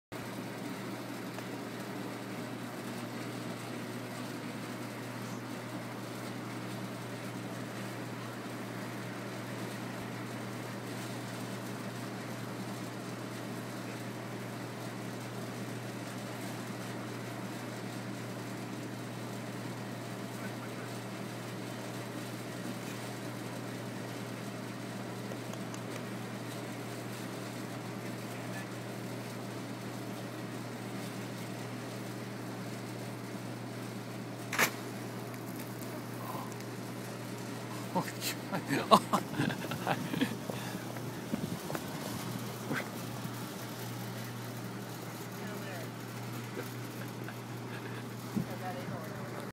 Water/Dry Ice Rocket
Simple mixture of water, and dry ice in which kinetic energy launches a 2-L bottle.